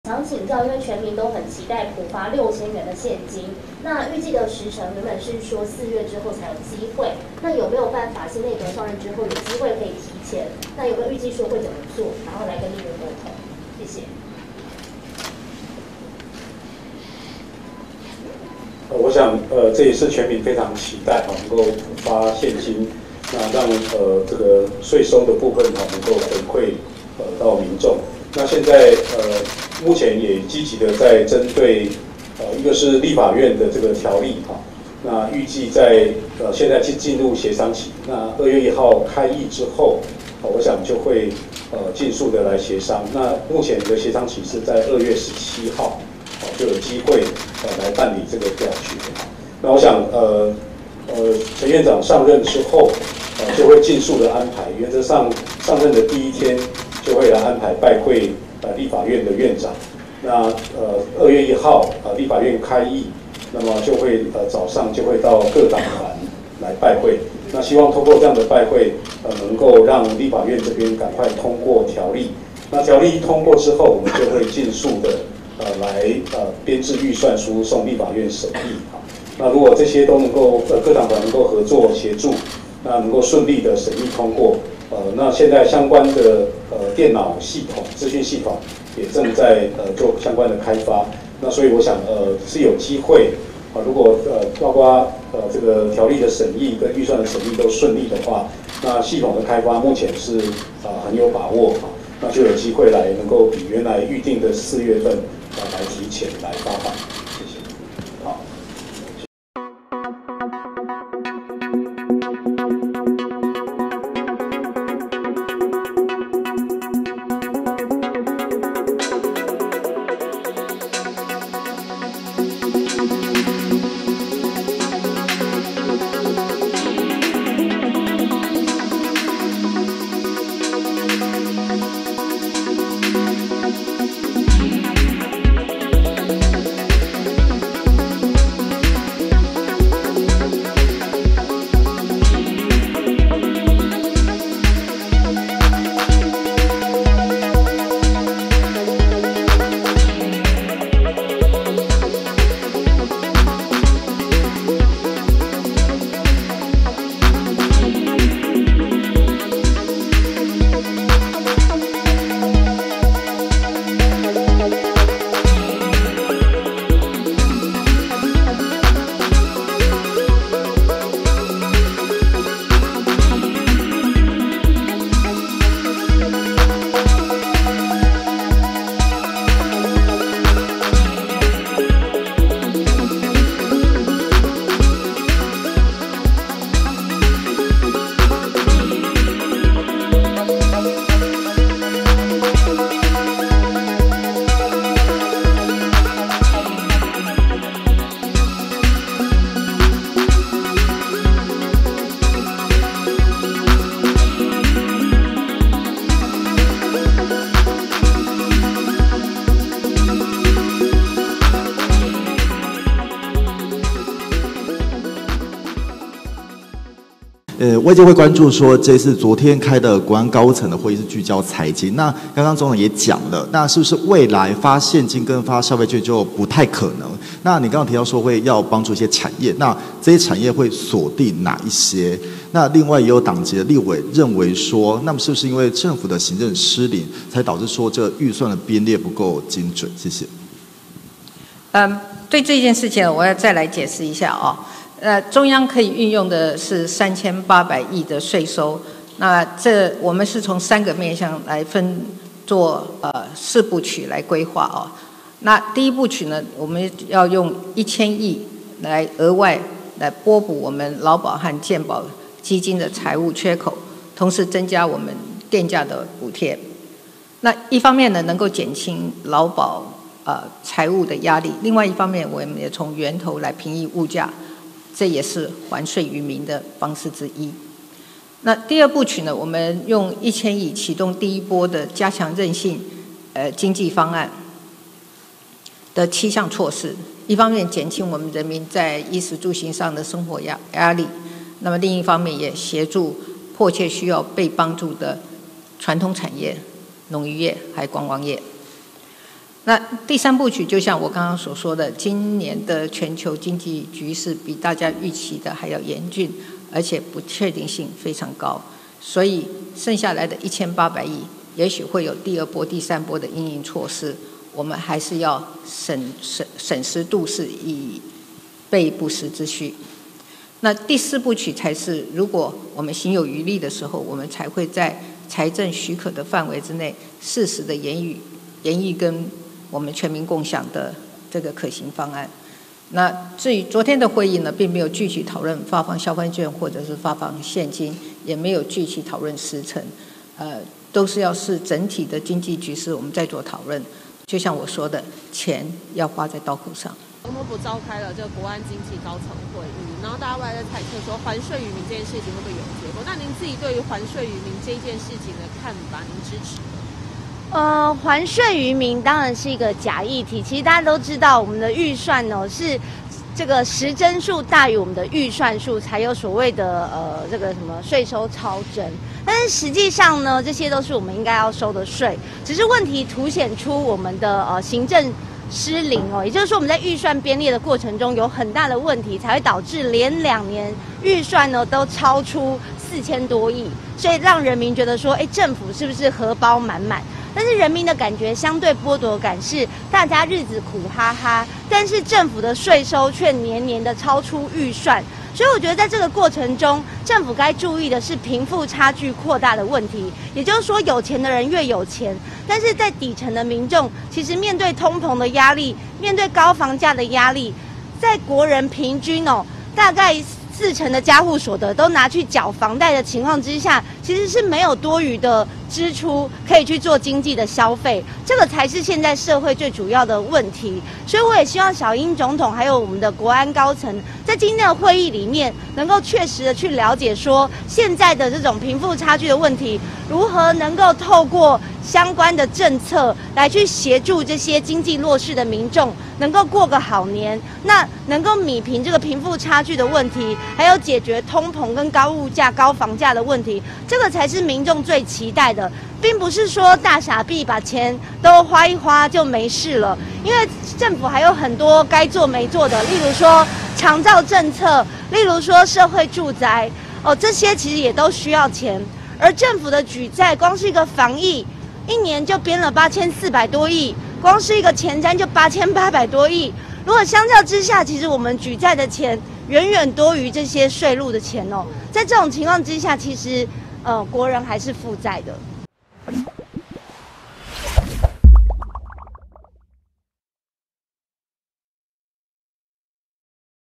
陳建仁上任要發6000元了？ 「最快時間點」曝光
準行政院長陳建仁明日將走馬上任，全民普發6000現金成為首要挑戰，但《疫後強化經濟與社會韌性及全民共享經濟成果特別條例草案》最快2月17日才表決，預計4月後才能發放。對此，行政院秘書長李孟諺稱，陳建仁明將拜會立法院長游錫堃、開議後將走訪各黨團，而相關電腦系統也在開發，若一切順利，有機會在4月前發放。